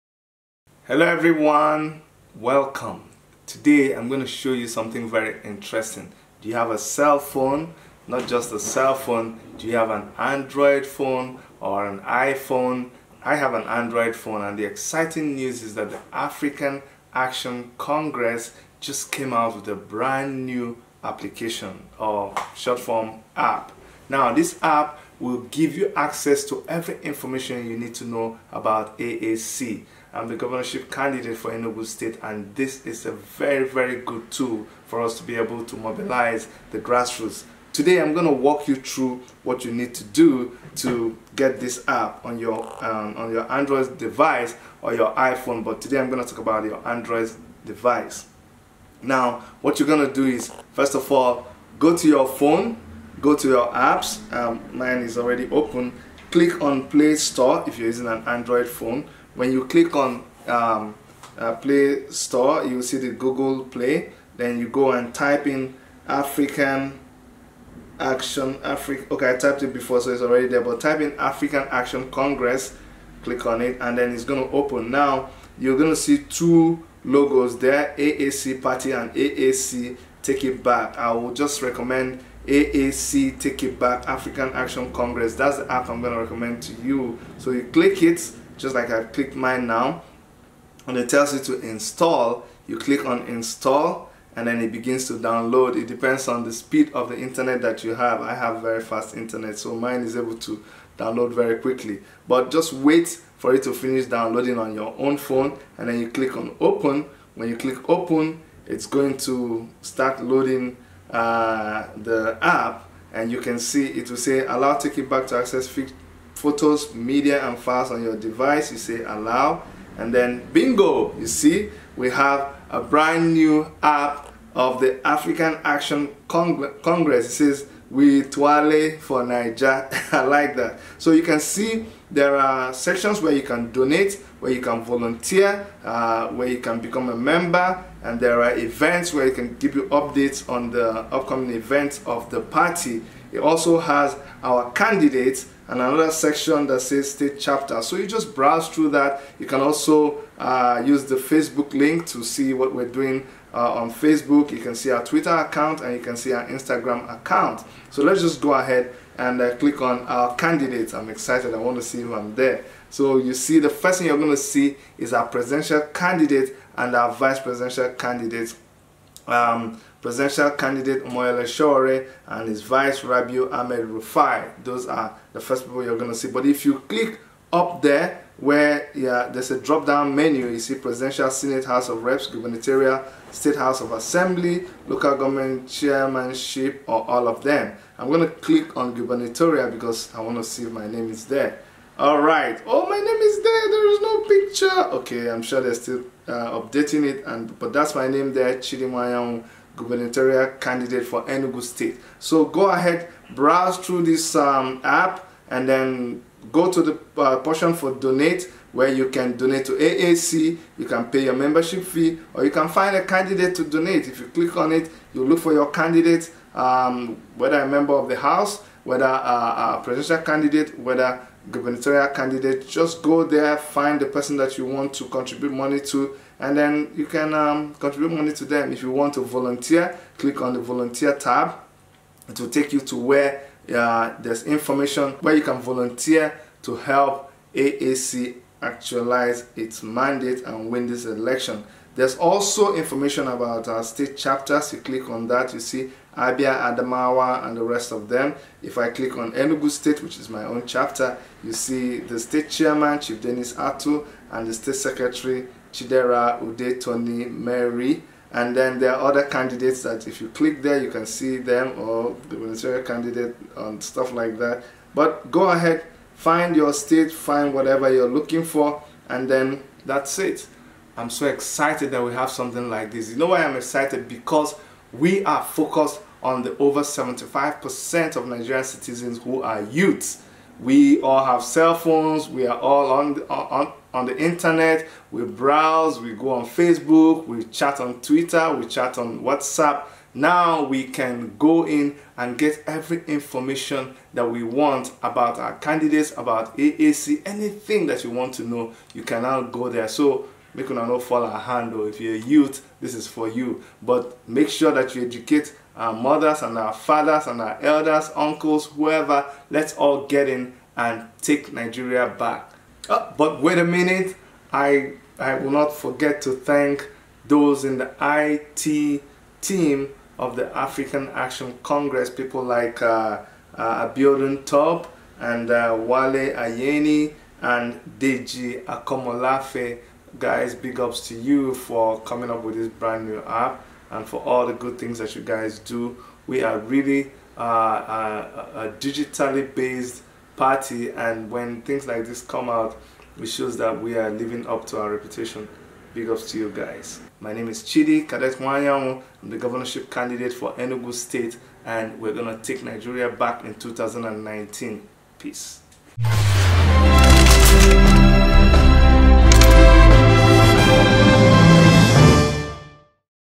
The AAC Take it Back mobile app - How it works!
The AAC Take it Back mobile app
There are many ways a political party can set itself apart from the norm. It can work tirelessly to be the transformation it wants to bring to the nation. It can open its doors to the nation; welcoming both the supporters and the naysayers until it forms an engine that works. A political party can take advantage of technology and harness it for growth and progress. ⠀
Technology is still a growing industry in Nigeria, with many smart phone users having limited access to any other device and the high cost of mobile data still presenting a challenge. It is for this reason that the third largest party in the country has decided to take the resource to the people and give them all the information they need about their country’s progress through the Take it Back Movement. ⠀
The app is symbolised by the now-familiar logo of two hands stretching towards the sky and can perform all the functions a supporter needs from his or her party. You can:⠀
1. Access & read the party’s constitution and manifesto.⠀
2. Register as a member of AAC party.⠀
3. Send a message to the party.⠀
4. Access the biographies and manifestos on all the aspirants and candidates for the different electoral positions under the party.⠀
5. Volunteer in any capacity to further the party’s mission and goals.⠀
7. Receive alert for party news, upcoming events and messages from aspirants and candidates.⠀
9. Easily access other useful links pertaining to the party.⠀
Take it Back and the AAC Party continue to be passionate about education, enlightenment and progress, and there are few parties who can lay claim to that in Nigeria today. ⠀
The AAC Take it Back mobile app – welcome to the future. ⠀